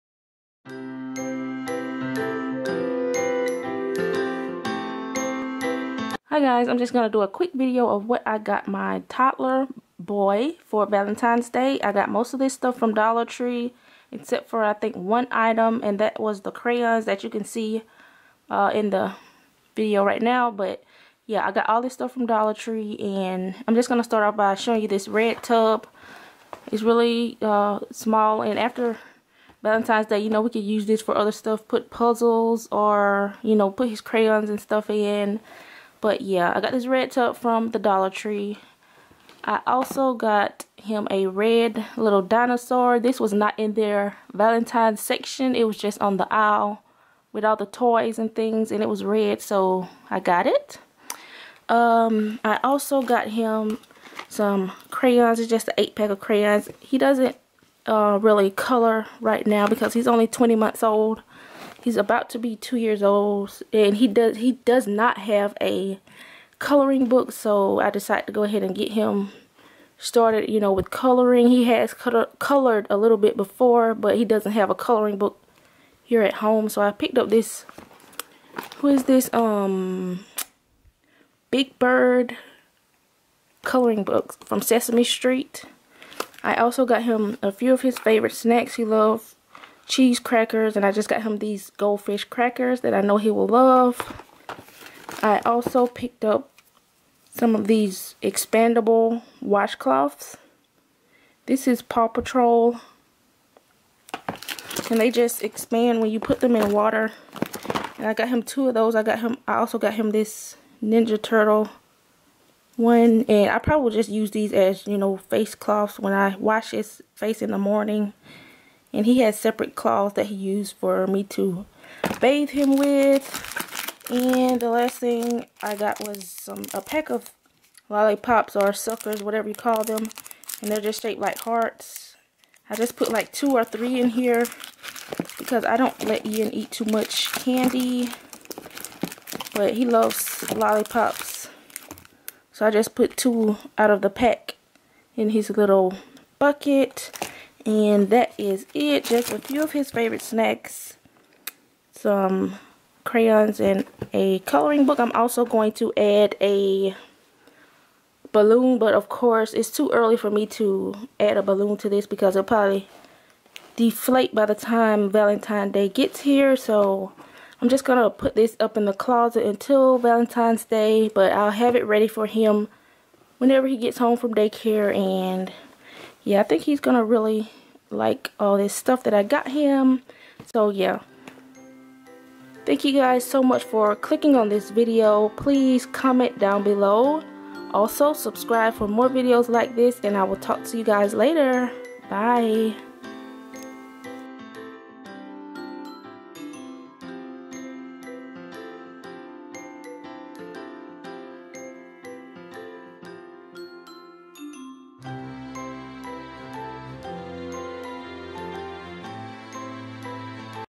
Toddler Valentine Gift Idea - 2018
These are a few items i picked up for Ian this Valentines day 2018!